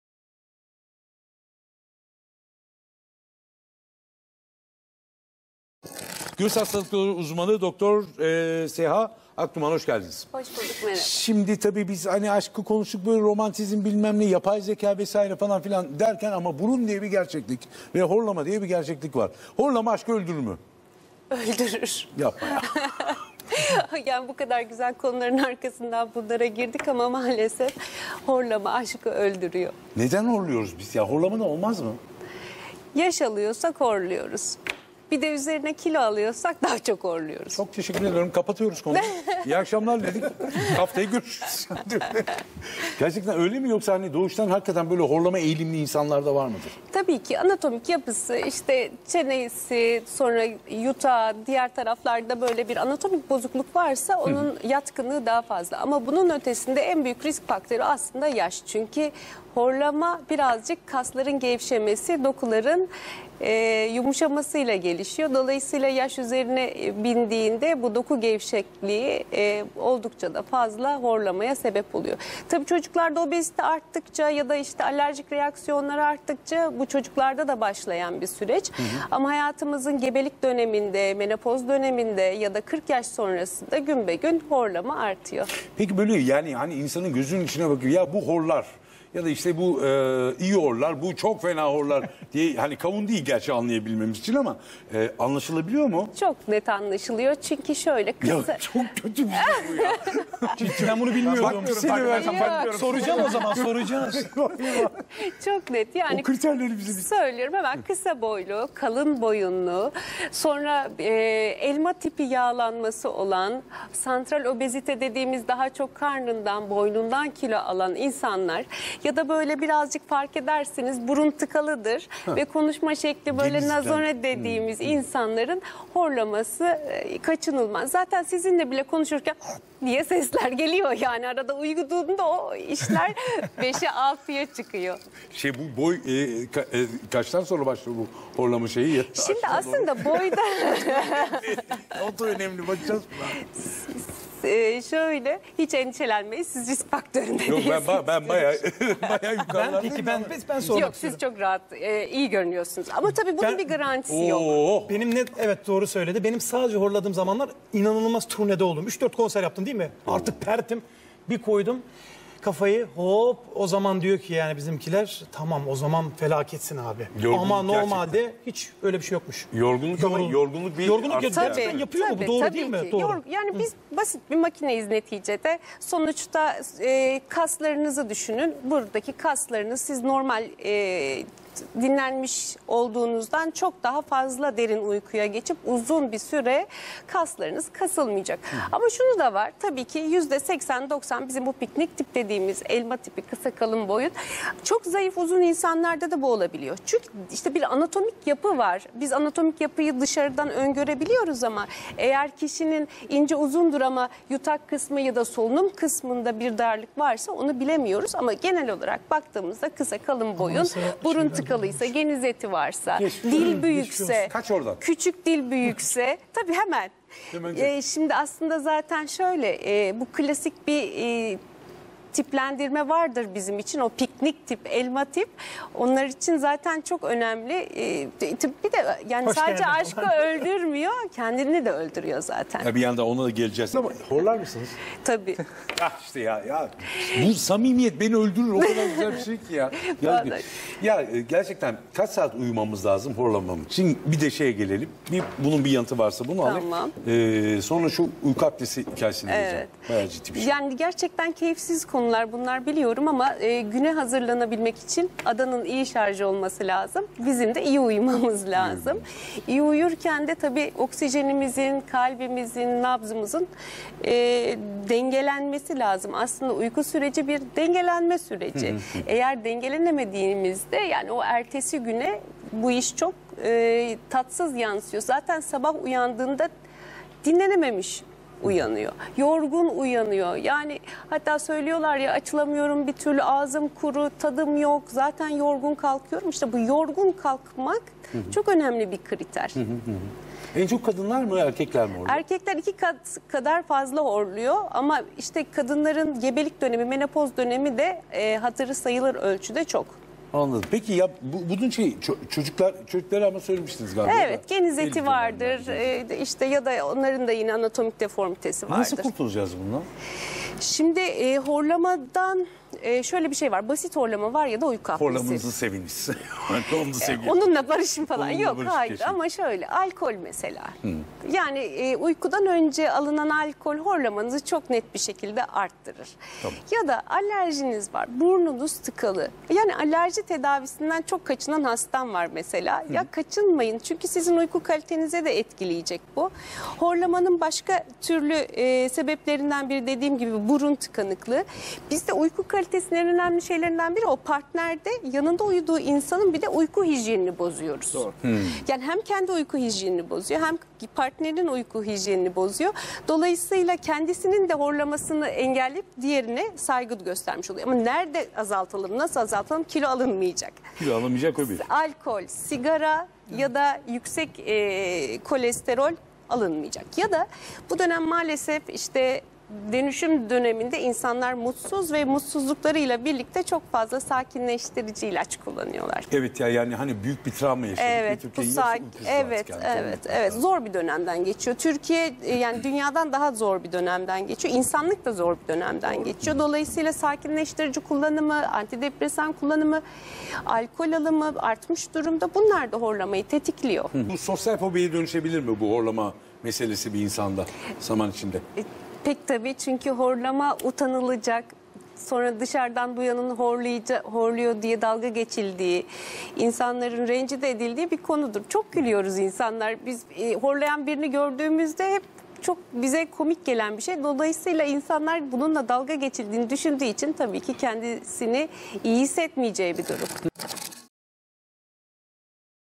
Horlama Nedenleri ve Tedavisi | Dr. Öğr. Ü. Seha Akduman
Yeditepe Üniversitesi Hastaneleri göğüs hastalıkları Uzmanı Dr. Öğr. Ü. Seha Akduman horlama nedenleri ve tedavisi hakkında bilgi verdi.